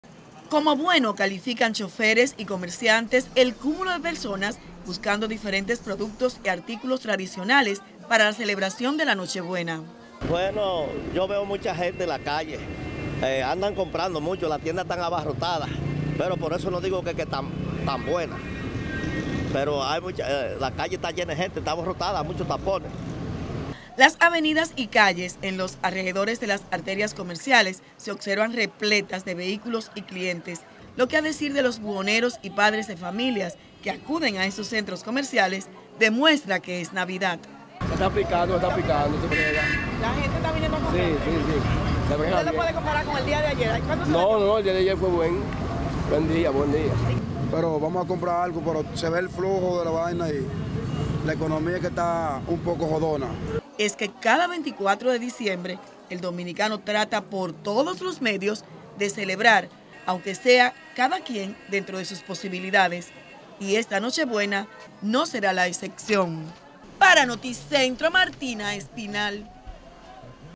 Se disparan actividades comerciales este 24 de Diciembre
Las actividades comerciales se dispararon este Martes 24 de Diciembre en el Gran Santo Domingo
Nuestra compañera Martina Espinal realizo un recorrido por diferentes arterias comerciales y nos informa